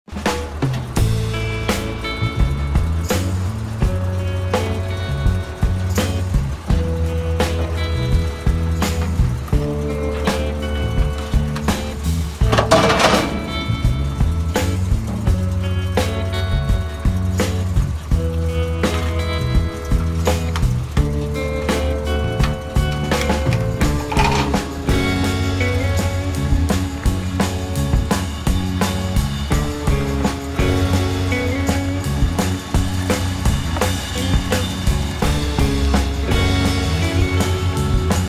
1969 Cadillac Seville unloaded
The Lone Ranger unloads a 1969 Cadillac Seville in Toms River, NJ. This car is being shipped across the country to Utah! Check out other cool cars we've loaded and unloaded!